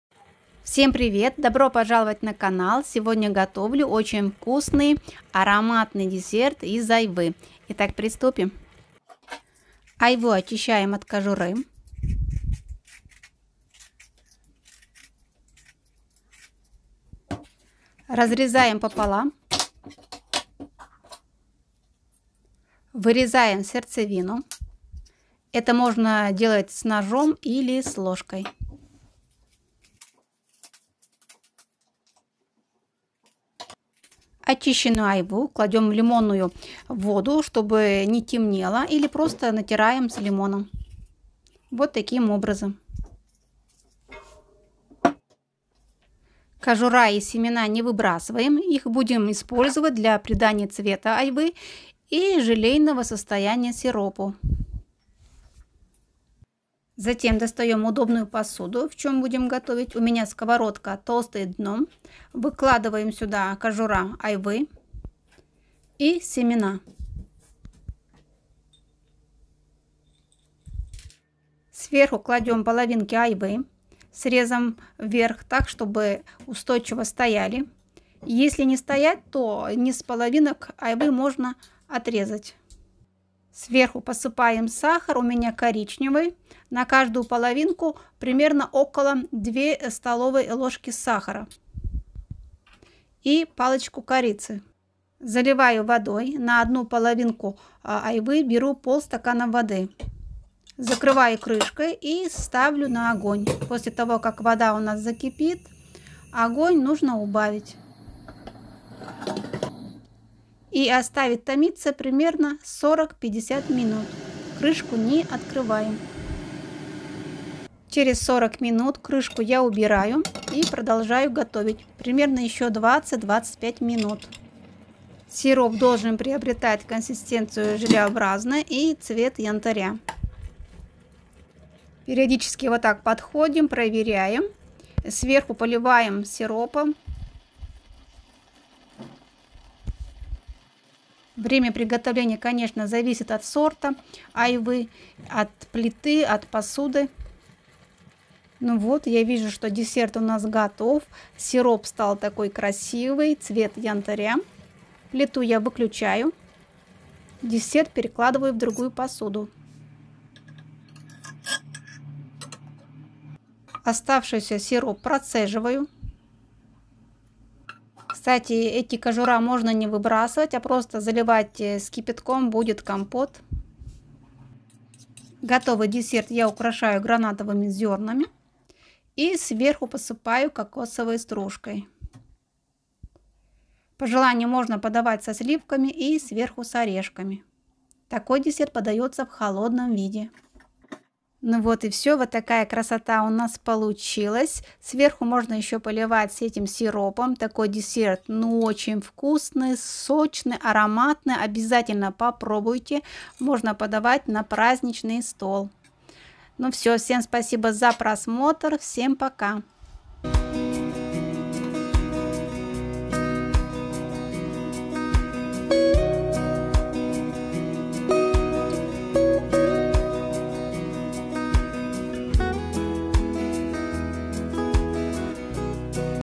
Айва. БЕЗУМНО ВКУСНЫЙ, АРОМАТНЫЙ, СОЧНЫЙ ДЕСЕРТ из АЙВЫ! Десерт на праздничный стол! Quince dessert
Легкий, безумно вкусный десерт из айвы! Можно подать со сливками, с орехами, с зернами граната...Рекомендую!!!

Ингредиенты:
Айва - 2 шт.
Сахар
Корица по желанию
На одну половинку айвы - 2- ст.л. сахара
На одну половинку  айвы - 1/2 стаканы воды ( 100 мл.)

Light, insanely delicious quince dessert! It can be served with cream, with nuts, with pomegranate seeds ... I recommend !!!

Ingredients:
Quince - 2 pcs.
Sugar
Cinnamon optional
For one half of quince - 2 tbsp. Sahara
For one half of quince - 1/2 glass of water (100 ml.)